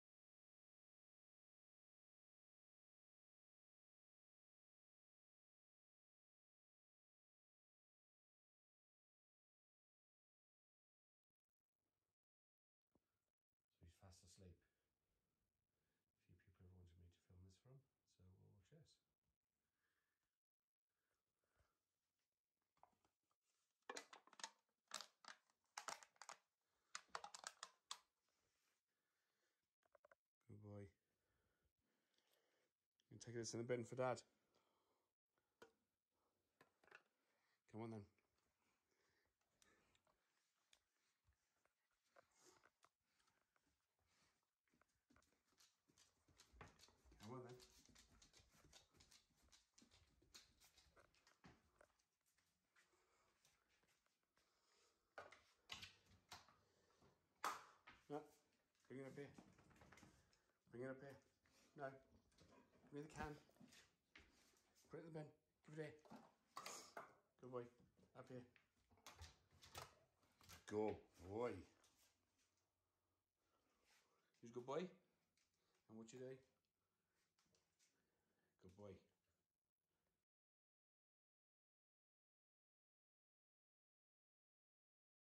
Buddy The Labrador Knows Just What To Do With My Empty Beer Tin!
This is our 10yr old Buddy the Labrador taking my empty beer can to the recycling as we tell him.
He always takes it to the kitchen sink and jumps up to hand it over, this is because the recycling is just next to the sink.

A few of my friends have asked recently to see this as they don’t believe he does it.

Fast asleep and he hears the can being crushed and he is up.
He also does empty crisp packets and actually drops that in the peddle bin.😎😎😎😎

He’s such a good dog fair play to him.👍🏼👍🏼👍🏼👍🏼👍🏼🐕🐕🐕🐕🐕